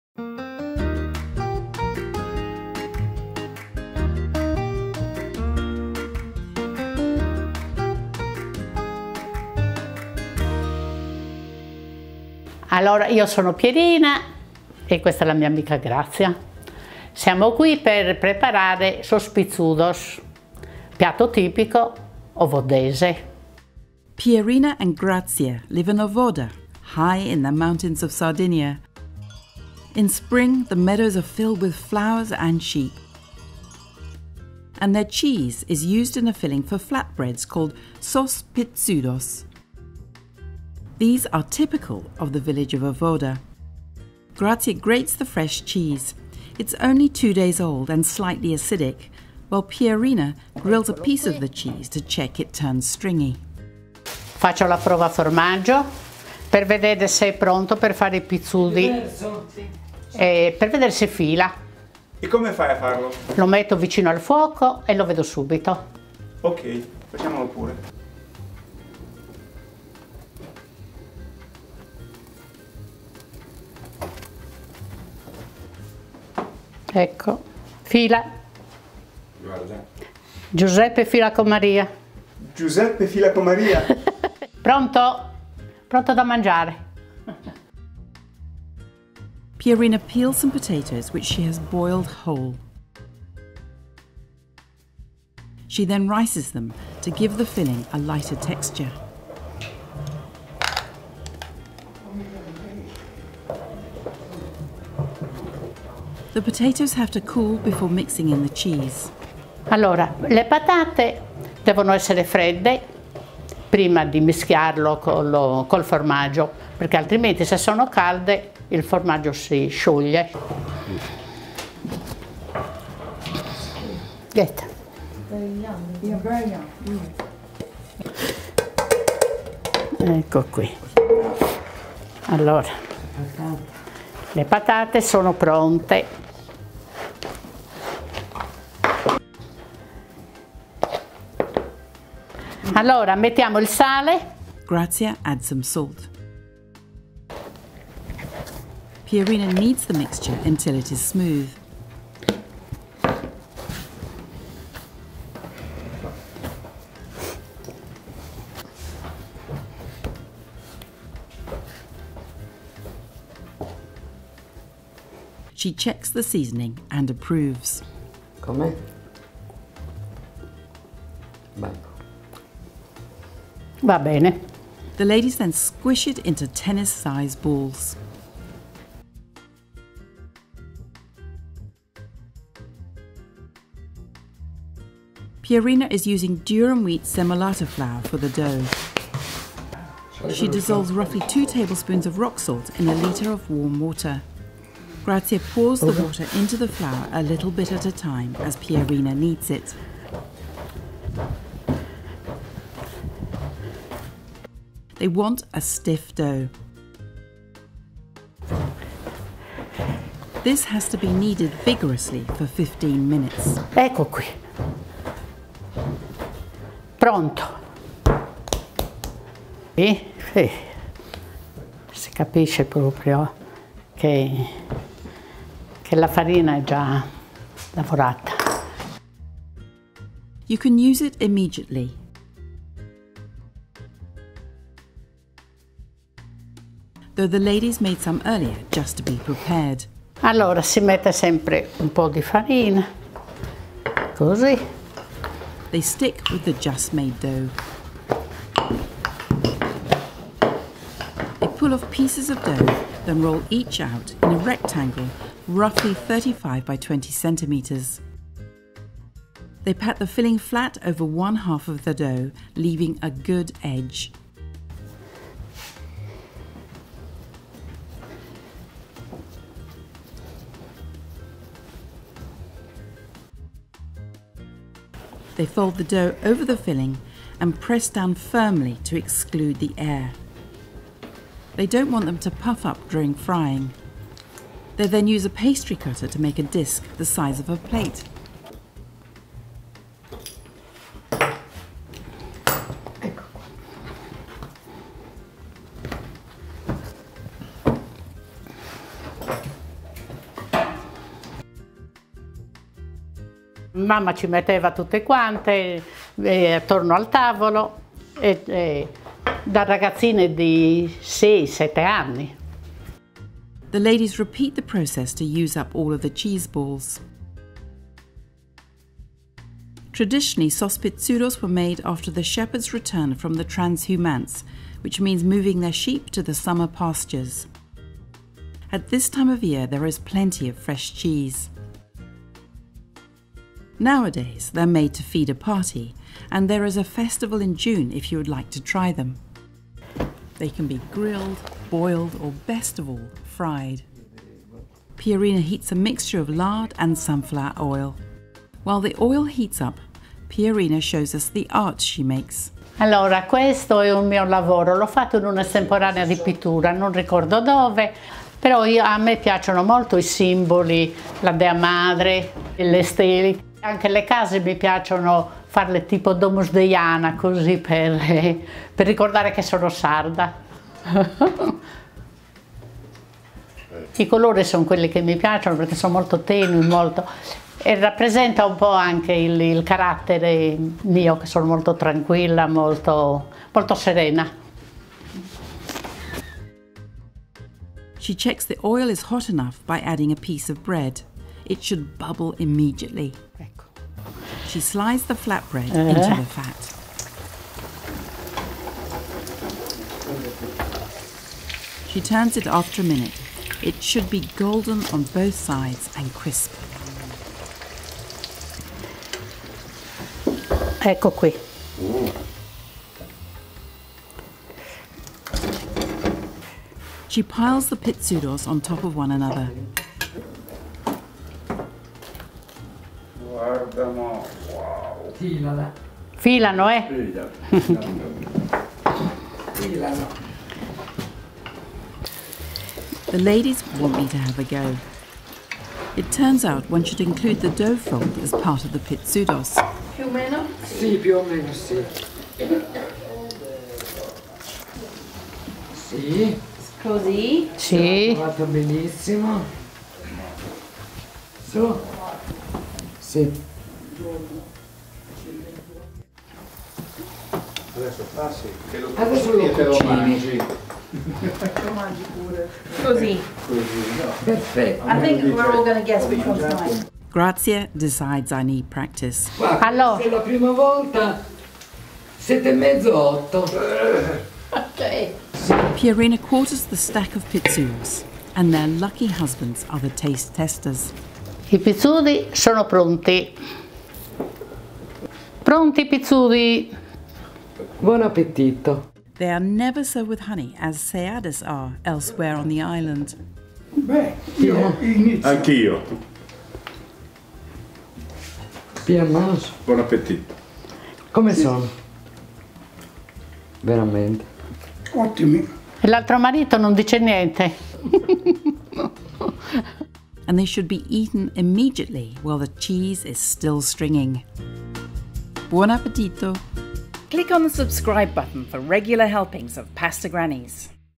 Enjoy a stringy cheese fried pastry from Sardinia called Sos Pitzudos! | Pasta Grannies
If you like your pastries crunchy, cheesy and gooey, then this is the recipe for you! Sos Pitzudos belong to a family of fried pastries one finds in Sardinia, taking advantage of the islands pecorino cheese. Pierina's recipe is as follows
500g durum wheat flour
250ml warm water in which you've dissolved half a teaspoon of crystal salt.
For the filling:
2 large old potatoes, boiled and riced (you could bake them too for a fluffier texture)
500g fresh pecorino cheese, grated, salt if necessary.
Plenty of oil for frying - lard is really good for creating a crisp pastry and Pierina mixes it with sunflower oil.
They should be stacked then quartered for serving.